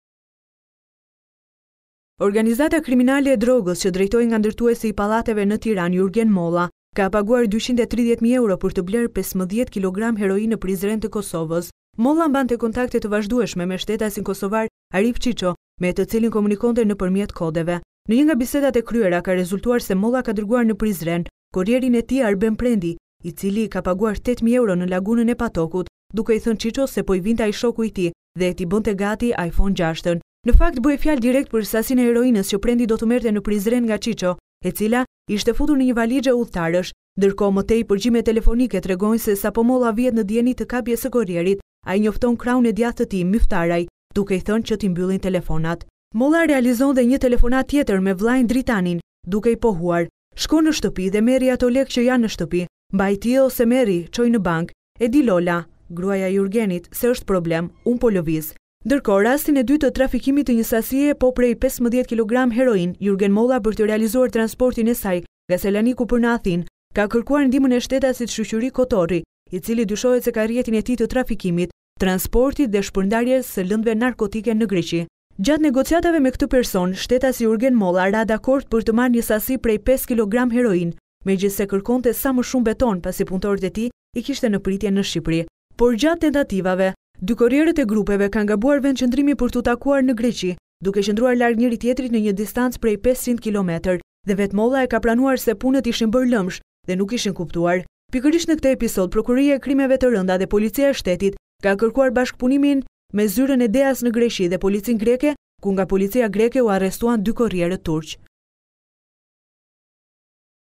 Report TV - Përgjimet e organizatës, kodet e bandës nga 'Iphone 6' te 'betoni'
Organizata kriminale e drogës që drejtohej nga ndërtuesi i pallateve në Tiranë Jurgen Molla ka paguar 230 mijë euro për të blerë 15 kg heroinë në Prizren të Kosovës.

Molla mbante kontakte të vazhdueshme me shtetasin kosovar Arif Çiço me të cilin komunikonte nëpërmjet kodeve. Në një nga bisedat e kryera ka rezultuar se Molla ka dërguar në Prizren korrierin e tij Arben Prendi të cilit i ka paguar 8 mijë euro në lagunën e Patokut, duke i thënë Çiços se po i vinte ai shoku i tij dhe duhet t’i bënte gati “Iphone 6”. Në fakt bëhej fjalë direkt për sasinë e heroinës që Prendi do merrte në Prizren nga Çiço, e cila ishte futur në një valixhe udhëtarësh.

Ndërkohë më tej përgjimet telefonike tregojnë se sapo Molla vihet në dijeni të kapjes së korrierit, ai njofton krahun e djathtë të tij Myftaraj, duke i thënë që t’i mbyllin telefonat. Molla realizon dhe një telefonatë tjetër me vëllain Dritanin, duke i pohuar “...Shko në shtëpi dhe merri ato lekët që janë në shtëpi, mbaji ti ose merri çoji në Bankë, e di Lola (gruaja e Jurgenit), se është problem. Unë po lëviz”.